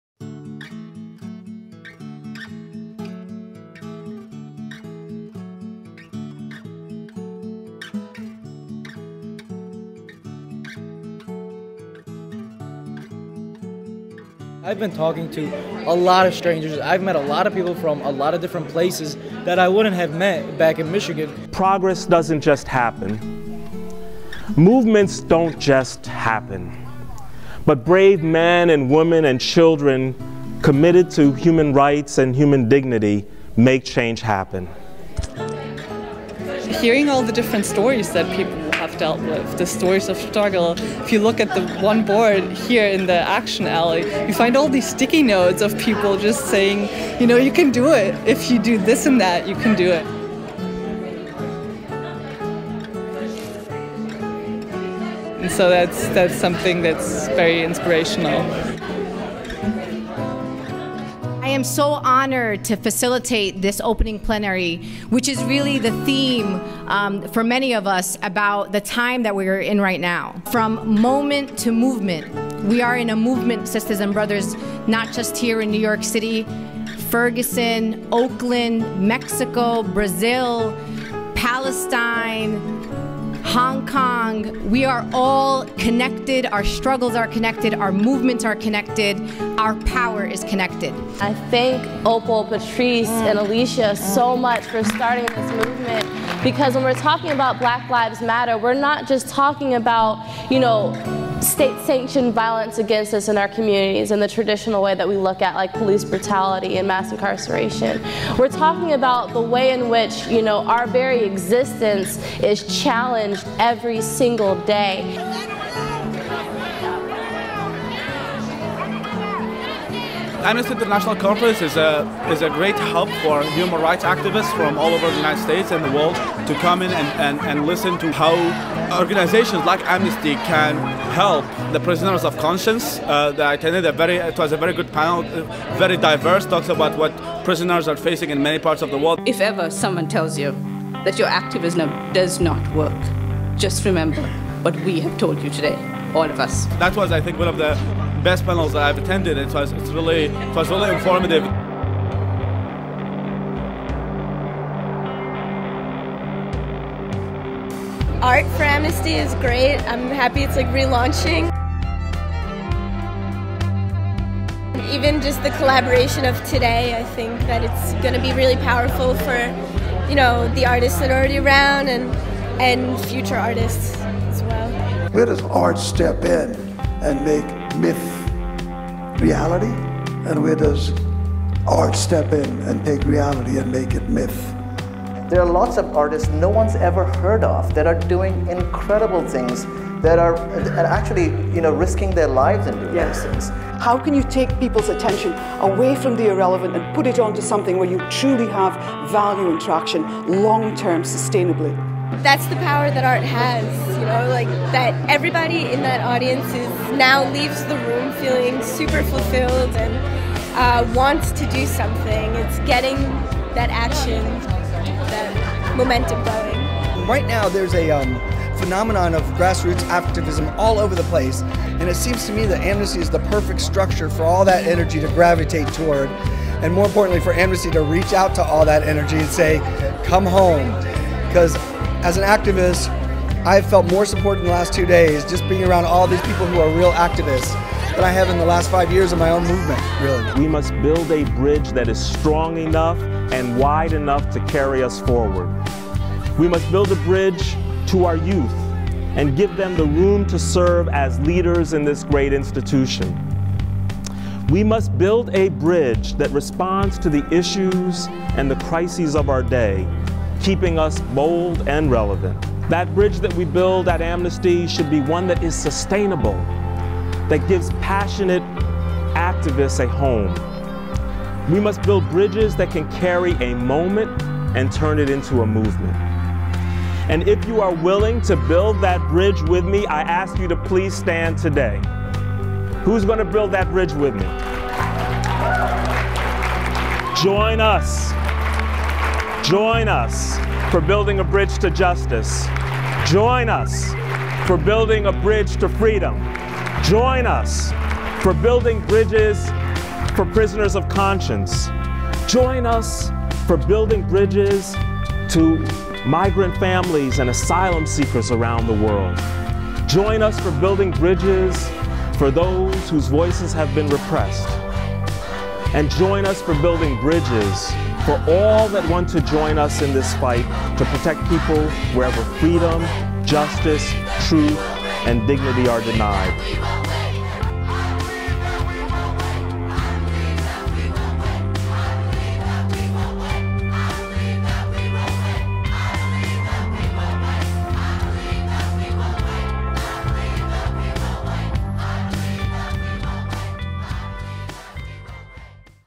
"From Moment to Movement": Building Bridges for Human Rights
On March 20-22, 2015, over 1,100 activists gathered in Brooklyn, NY for Amnesty International USA’s 50th Annual General Meeting. The theme of the conference was "From Moment to Movement," reflecting the spark of change that can begin in an instant and reverberate for years.

The conference featured the re-launch of Art for Amnesty in the US; an incredible human rights action on the Brooklyn Bridge to raise awareness about three New Yorkers recently killed by police; inspiring plenaries with organizers from the protests in Ferguson, Hong Kong and Mexico; panel discussions with former Prisoners of Conscience, youth activists and seasoned human rights organizers; and many more networking and action opportunities where activists created their own spaces and strategies to build our movement!

“We must build bridges that can carry a moment and turn it into a movement. That bridge that we build at Amnesty should be one that is sustainable, that gives passionate activists a home. And if you are willing to build that bridge with me, I ask you to please stand. And join us for building bridges for all those who want to join us in this fight to protect people wherever freedom, justice, truth and dignity are denied!” ~Steven W. Hawkins, Amnesty International USA executive director

Featured in this video (in order of appearance):

- Mohamed – activist/attendee

- Steven W. Hawkins - Amnesty International USA executive director

- Lisa – volunteer

- Linda Sarsour – Arab American Association of New York executive director

- Ciara Taylor, Dream Defenders political director

- Faisal - activist/attendee

- Ronnate Asirwatham, activist and wife of Prisoner of Conscience J.S. Tissainayagam

- Lynn – activist/attendee

- Jie-Song Zhang – musician, Huffington Post writer, and author

- Brandan Odums – artist/founder & director of 2-Cent Entertainment

- Harry Belafonte – philanthropist, humanitarian, activist, actor, singer and 2013 Ambassador of Conscience awardee

- Nusrat Durrani – general manager and senior vice president of MTV World, creator and executive producer of the ‘Rebel Music’ series

- Annie Lennox – human rights activist, philanthropist, singer and songwriter

- Todd – activist/attendee